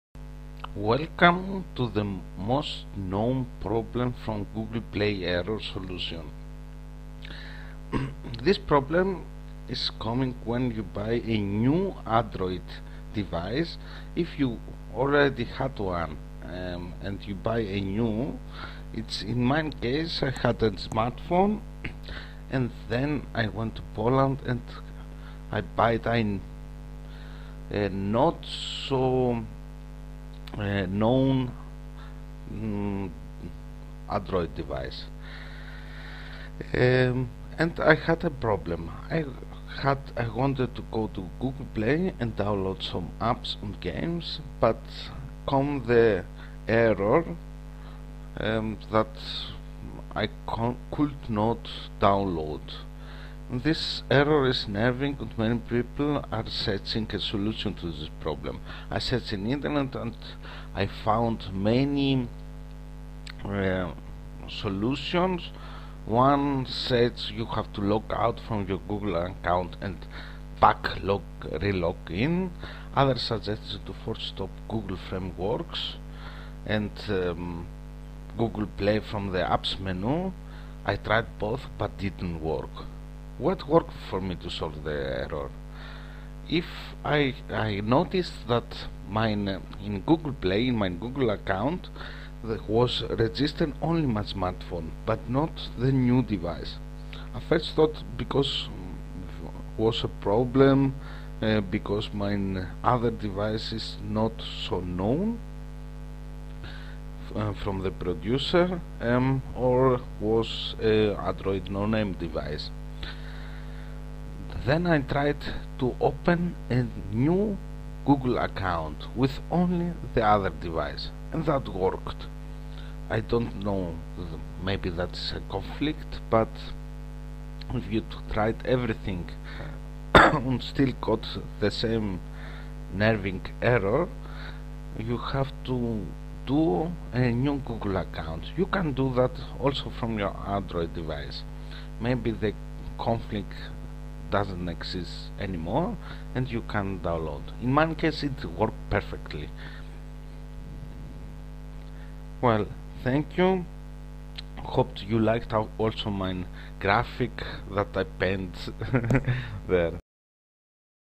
Tutorial how to fix Google Play Error [RPC:S-5:AEC-0]
Google Play "Error retrieving information from server. [RPC:S-5:AEC-0]" solution!
If you recently bought an new android smartphone or android tablet und tried to install apps or games from Google play store and keeps coming the error message , error by downloading   [RPC:S-5:AEC-0] its nerving and many people are searching a solution to this problem.
I had the same problem as one cheap android tablet recently buyied and tried to use Google play. Then i searched the internet to see what's the problem   [RPC:S-5:AEC-0]  solution. many said you have to erase your Google account and again relog in , others suggested to force stop Google frameworks and Google play from the apps menu. Both didn't work.
But I found the solution to the most known problem on google play error and since then I have definitely solved and can since then with my "no name" android device use google play store without errors. See the video of my own case story and solution or also visit my new android blog article (for the solution written in case you did not understand the video tutorial) there

Sorry for my bad English but really the solution is working 100% !!If it worked the plz thumbs up and like the video!